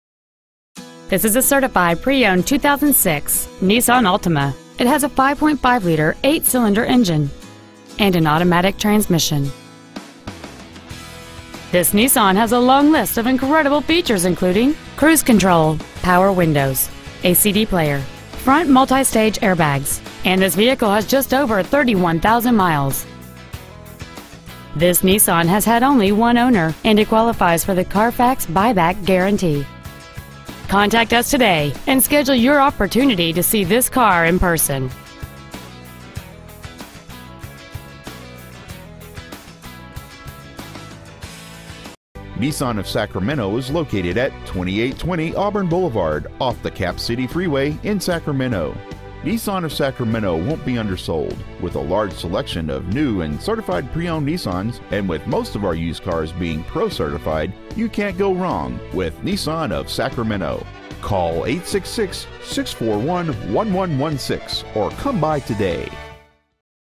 Pre-Owned 2006 Nissan Altima Sacramento CA
Year: 2006
 Make: Nissan
 Model: Altima
 Engine: v6 3.5 liter
 Trans.: automatic
 Exterior: red
 Miles: 31,406
 Interior: beige

 Used 2006 Nissan Altima Sacramento CA